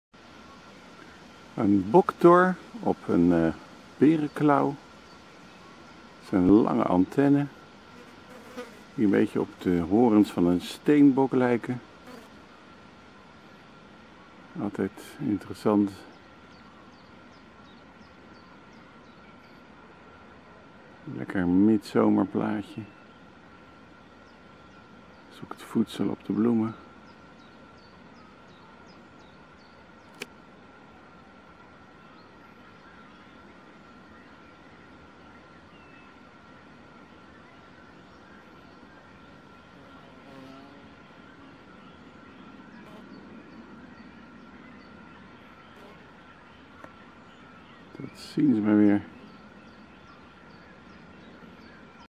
20210718 6 Rockanje bij Breede Water Zwarttip smalboktor op bereklauw
De Zwarttip-Smalboktor, Paracorymbia fulva, hier lekker onder het stuifmeel aan het rondscharrelen op een bloem van Bereklauw. Deze boktor is in ons land in het Zuiden vrij algemeen, en in het Noorden juist vrijwel afwezig.